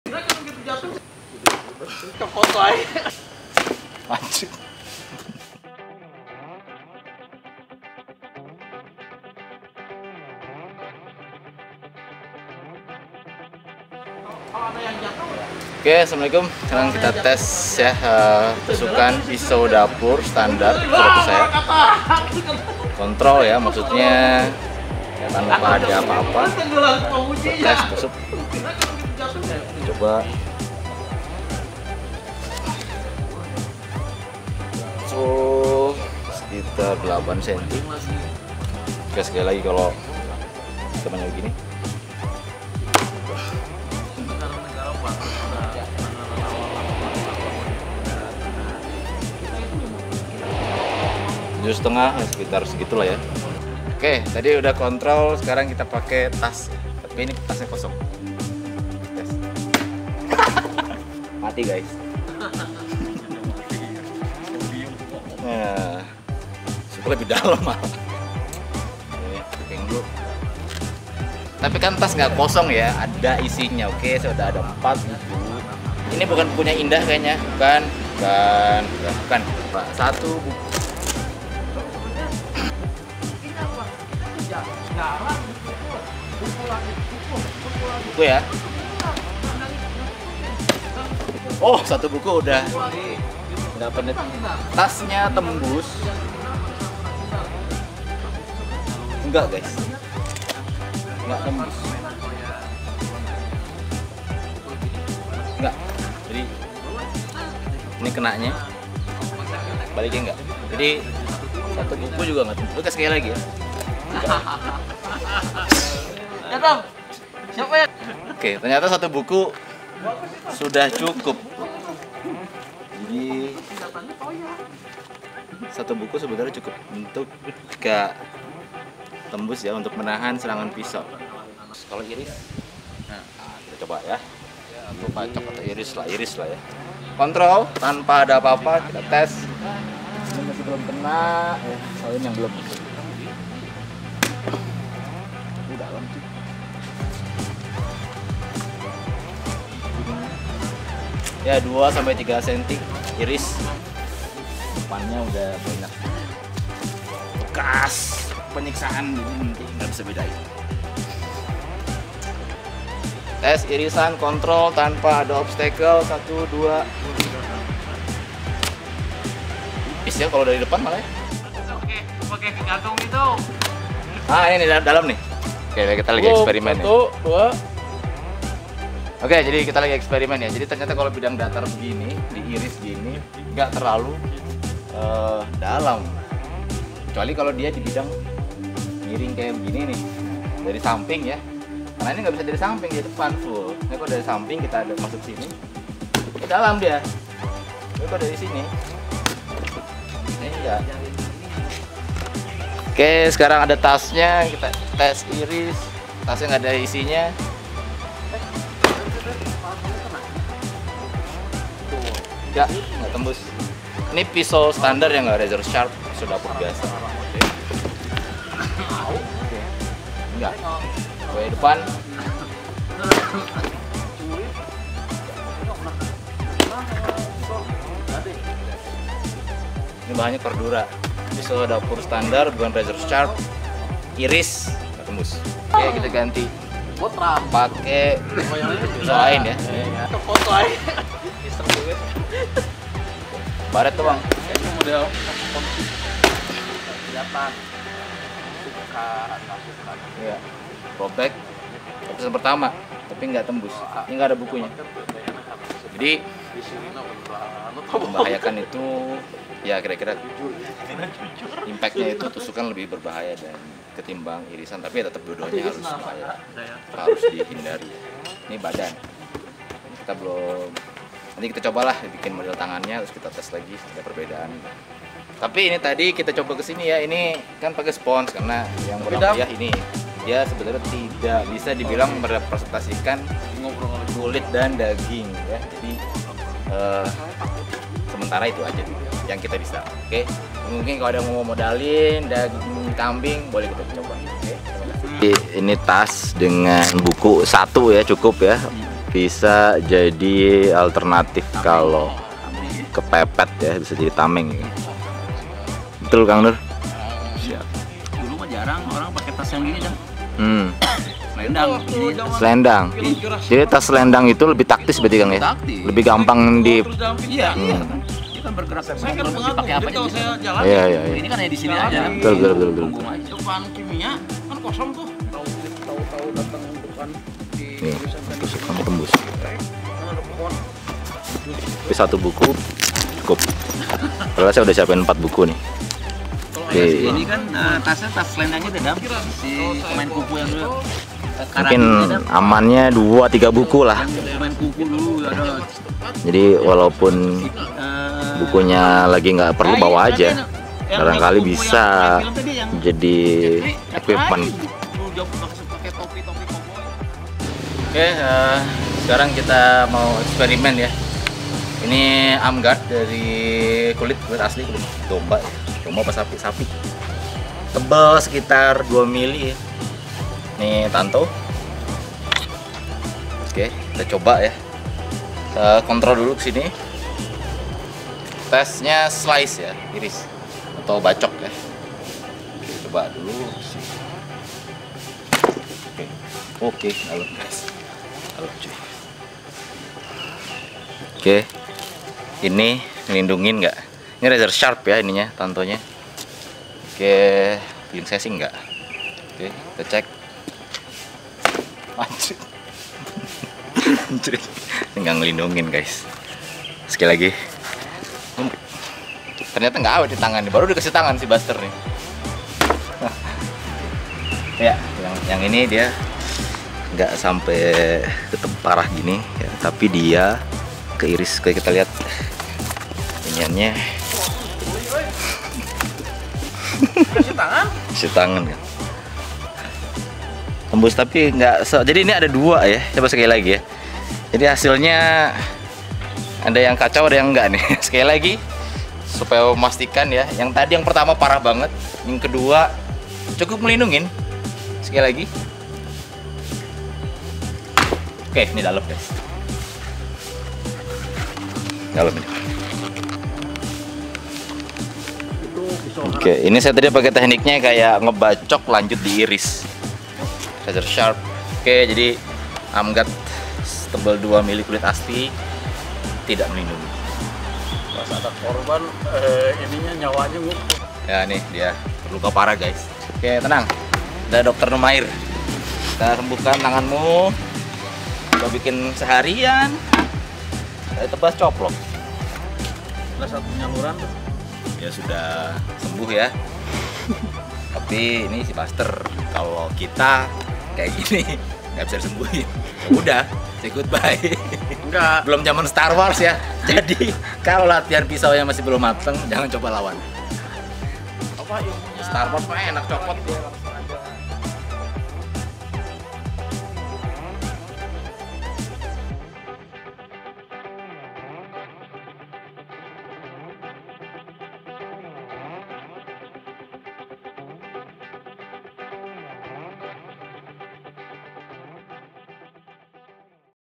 eksperimen impact serangan pisau
Tema latian kali ini
- tes impact tusukan dan irisan pisau
- dampak perlindungan beberapa barang